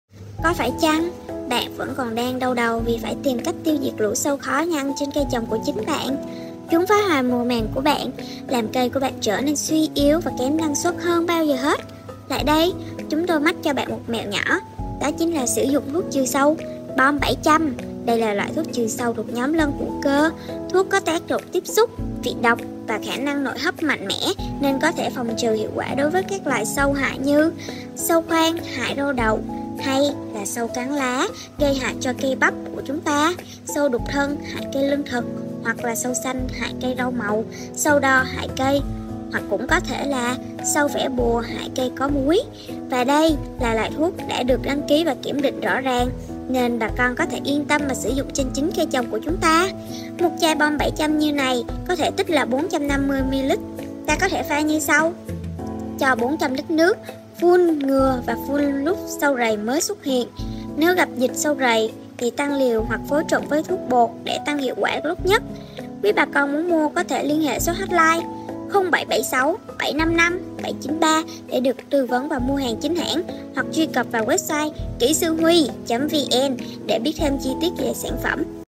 BOM 700 THUỐC TRỪ SÂU BISECTOR 500EC – BỌ TRĨ – RỆP SÁP – RẦY GÂY HẠI TRÊN CÂY TRỒNG - 0345.37.88.39
BOM 700 ATT THUỐC TRỪ SÂU BISECTOR 500EC TRỪ

BỌ TRĨ – RỆP SÁP – RẦY GÂY HẠI TRÊN CÂY TRỒNG

BOM 700 ATT BẢO VỆ THỰC VẬT ĐỒNG NAI
THÀNH PHẦN BOM 700 ATT
Dimethoate 400g/l

Fenobucarb………..100g/l

Phụ gia đặc biệt….500g/l

CÔNG DỤNG BOM 700 ATT
BOM 700 ATT là  thuốc trừ sâu thuộc nhóm lân hữu cơ, thuốc có tác động tiếp xúc, vị độc và khả năng nội hấp mạnh nên phòng trừ hiệu quả đối với các loài chích hút như rệp, nhện đỏ, bọ trĩ, bọ xít, rầy xanh, sâu vẽ bùa,….trên cây sầu riêng và nhiều loại cây khác.

BOM 700 ATT Thuốc được đăng ký trừ bọ trĩ, bọ xít hôi/lúa, sâu khoang/lạc.

Rất độc với kiến và ruồi

HƯỚNG DẪN SỬ DỤNG BOM 700 ATT
LÚA:

Bọ trĩ: Liều lượng: 1.0 lít/ha

Bọ xít hôi: Liều lượng: 1.0 lít/ha

Cách dùng: Lượng nước 400 – 500 lít/ha

Phun thuốc khi mật độ sâu khoảng 3-5 con/la

LẠC: Sâu khoang
Liều lượng: 0.9 lít/ha

Cách dùng: Lượng nước 400 – 500 lít/ha. Phun thuốc khi sâu tuổi 1 – 2 mật độ khoảng 1 – 2 con/cây.